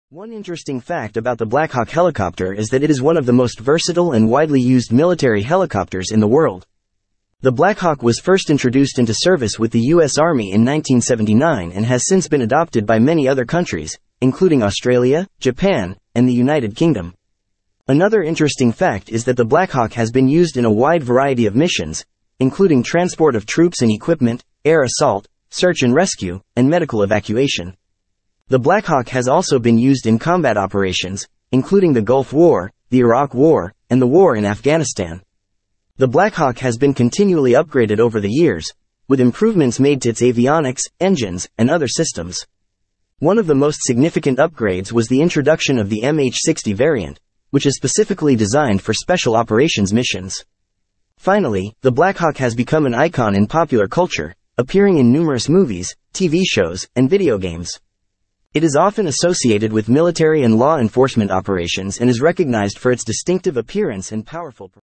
Blackhawk Helicopter
One interesting fact about the Blackhawk helicopter is that it is one of the most versatile and widely used military helicopters in the world. The Blackhawk was first introduced into service with the US Army in 1979 and has since been adopted by many other countries, including Australia, Japan, and the United Kingdom.

Another interesting fact is that the Blackhawk has been used in a wide variety of missions, including transport of troops and equipment, air assault, search and rescue, and medical evacuation. The Blackhawk has also been used in combat operations, including the Gulf War, the Iraq War, and the War in Afghanistan.

The Blackhawk has been continually upgraded over the years, with improvements made to its avionics, engines, and other systems. One of the most significant upgrades was the introduction of the MH-60 variant, which is specifically designed for special operations missions.

Finally, the Blackhawk has become an icon in popular culture, appearing in numerous movies, TV shows, and video games. It is often associated with military and law enforcement operations and is recognized for its distinctive appearance and powerful performance.